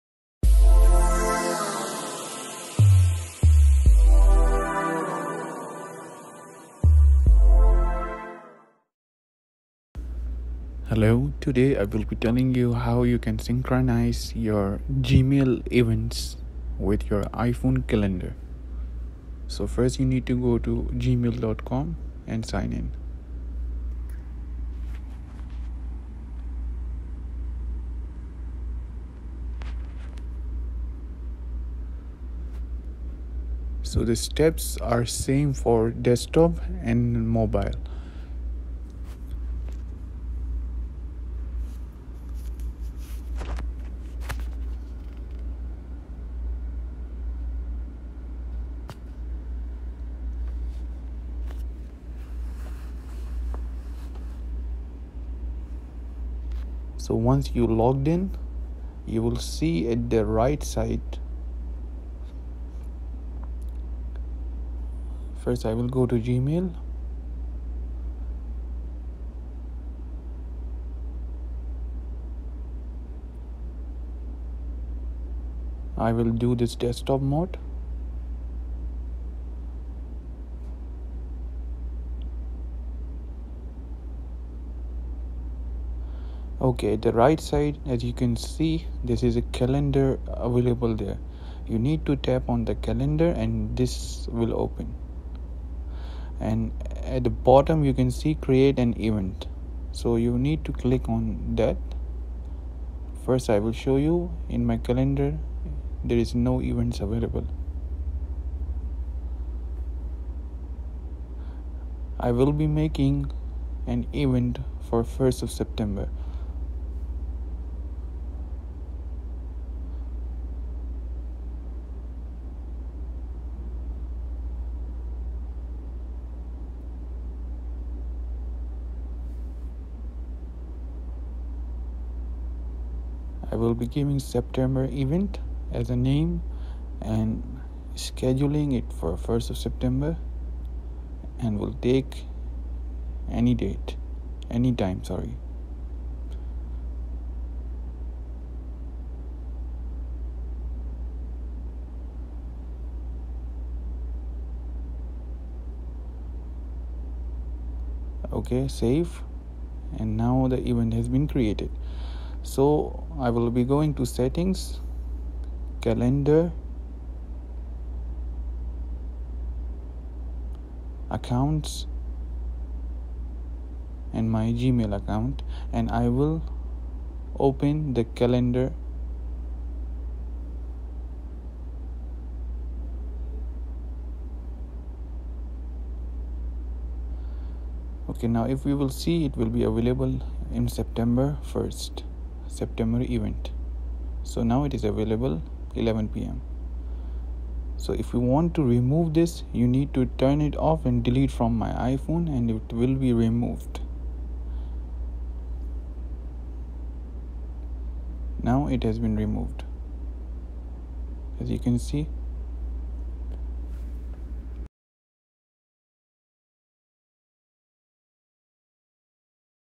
How to Use Gmail + Keep Notes + Google Calendar Together
In this I have shown how to connect your email events with Iphone calendar  so that you can be updated with your events

and